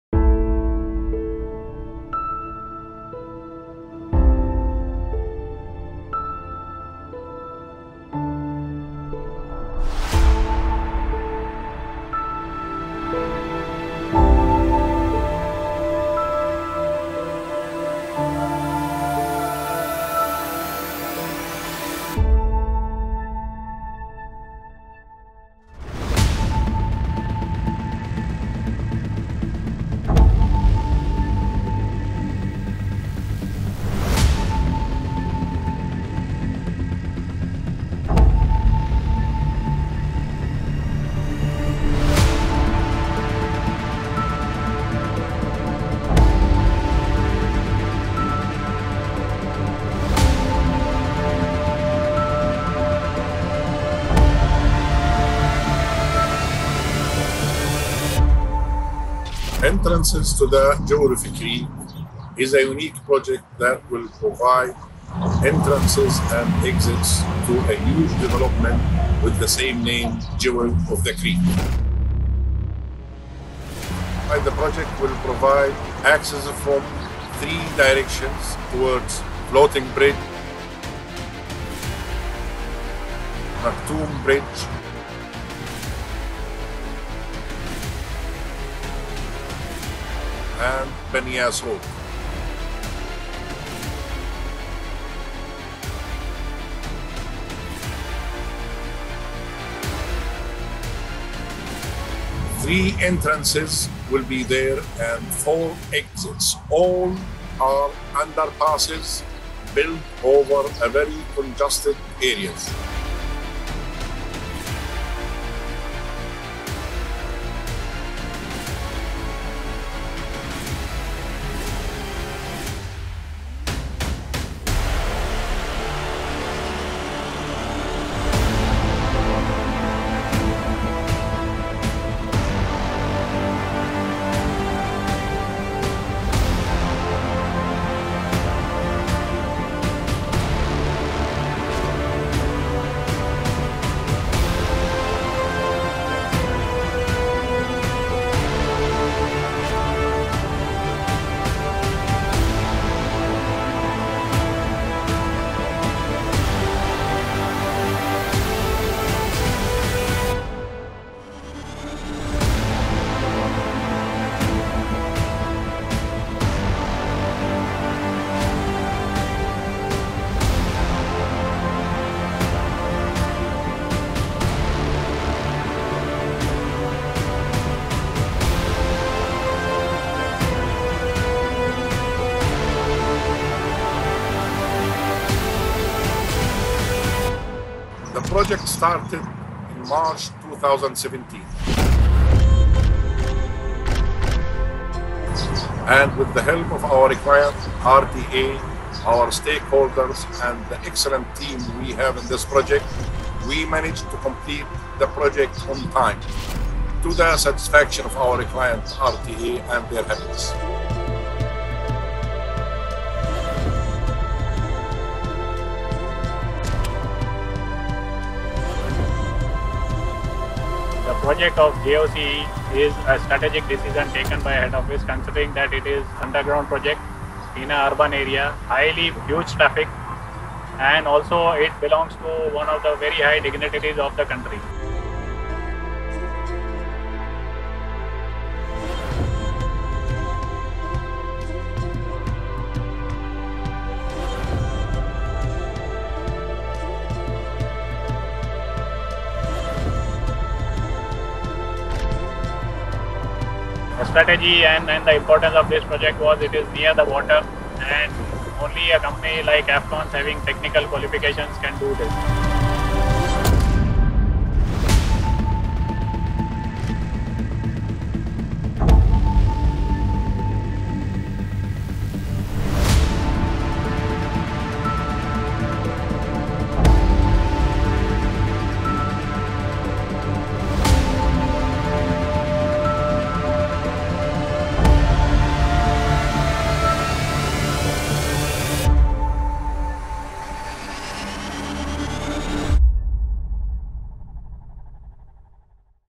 Entrances To The Jewel Of The Creek
Afcons has constructed seven exclusive entrance and exit ways to The Jewel Of the Creek (JOC), one of Dubai's most fascinating urban infrastructure projects. The seven underpasses are situated in the heart of Dubai - Deira City Centre area - that is buzzing with traffic. The project was commissioned by the Roads and Transport Authority of Dubai (RTA) that would connect JOC with the main road of Deira through new routes with underpasses. The design and uniqueness of the project makes it a landmark amidst Dubai's cityscape, adding prominence to the city, and the tourist profile of the area.